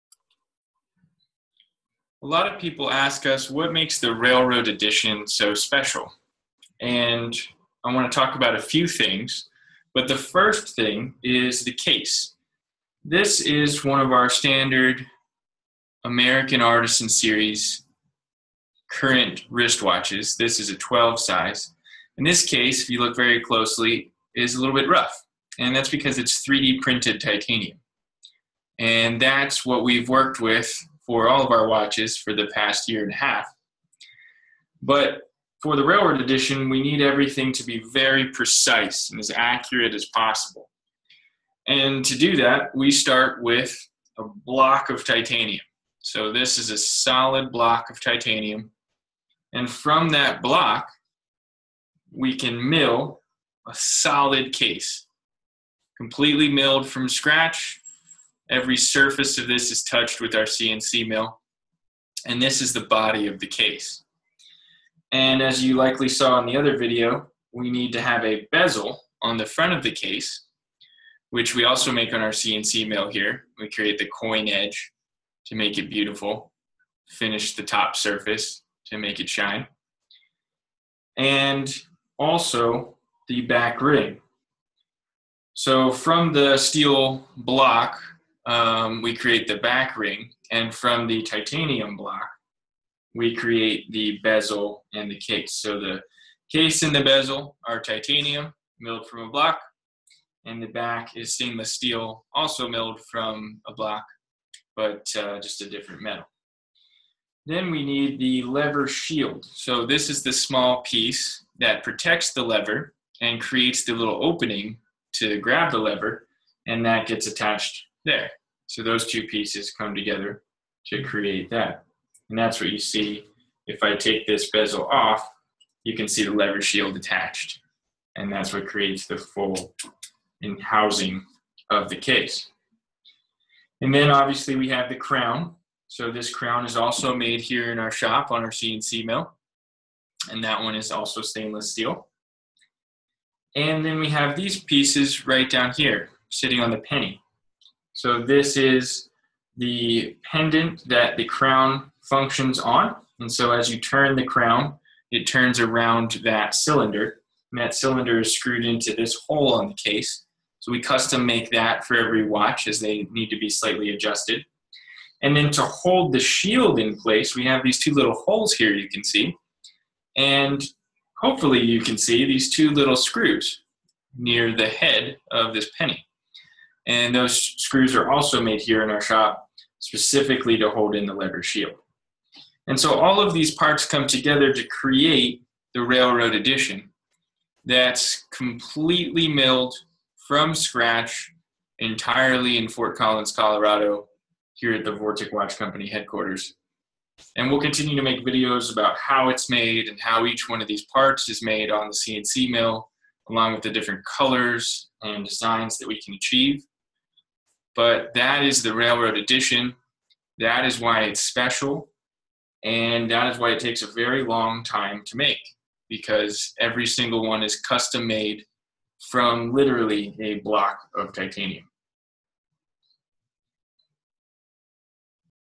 How It's Made - The Railroad Edition Components
In this video, R.T. Custer, Vortic's CEO, talks about what the Railroad Edition is made of, where all the parts come from, how those parts are made, and the steps of the manufacturing process.